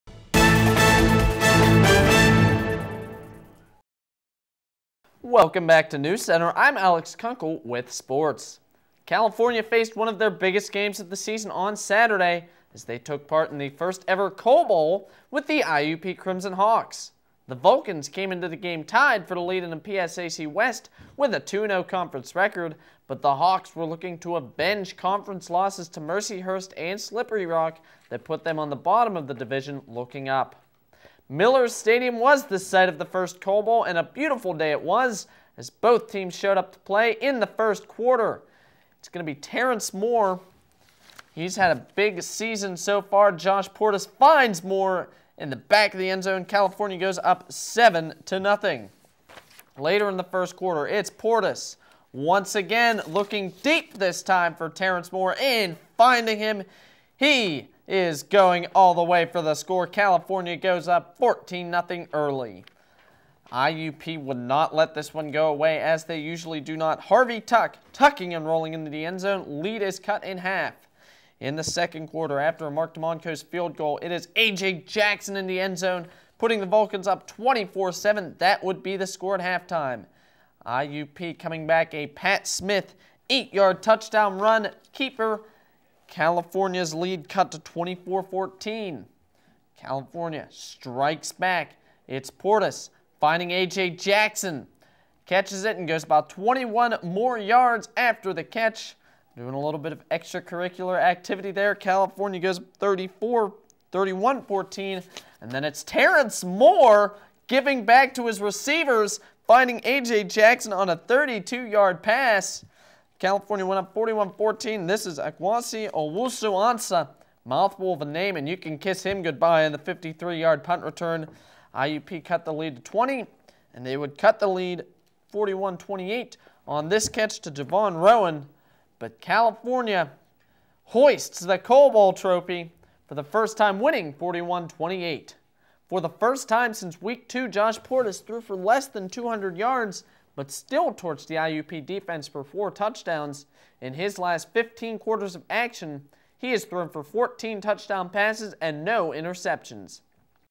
IUP vs. California Highlights
California travels to Indiana to take on the Crimson Hawks, Alix Kunkle has the highlights.